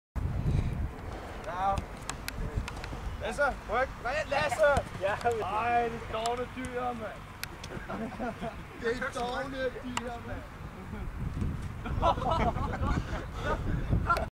Lasses Stunt
Lasse der forgæves forsøger at fange René the STAR PLAYER!